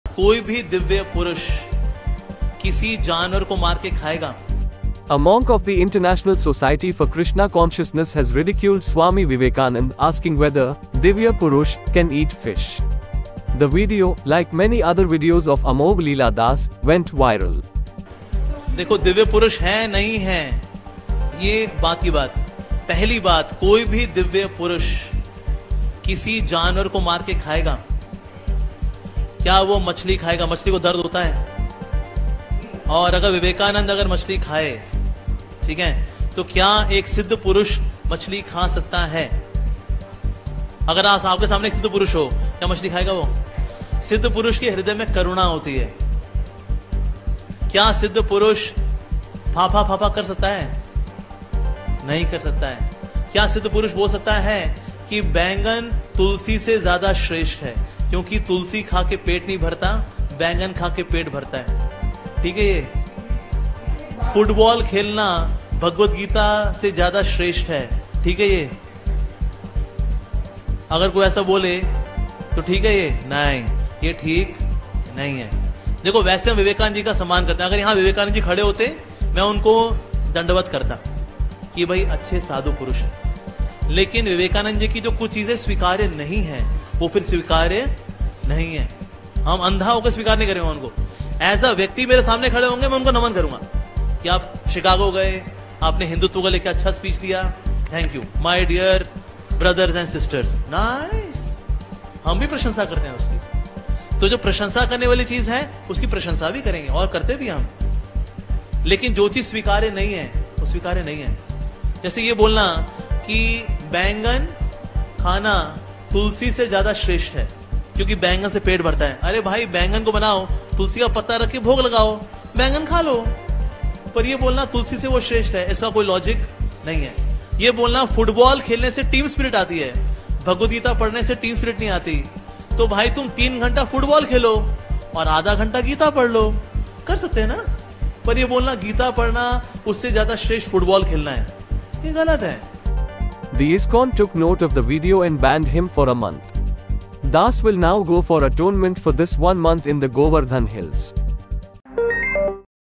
Why ISKON Monk Amogh Lila Das Was Banned?
A monk of the International Society for Krishna Consciousness has ridiculed Swami Vivekananda asking whether a 'divya purush' can eat fish.